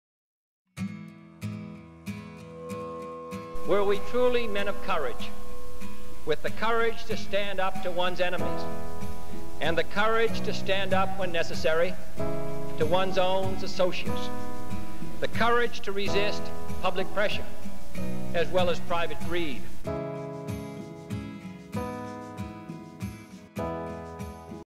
Submit your nomination for the Profile in Courage Award
Throughout history, brave leaders have inspired us in times of uncertainty and united us in moments of division—altering the fabric of society for the greater good.

Honor their legacy by nominating a leader for the Profile In Courage Award: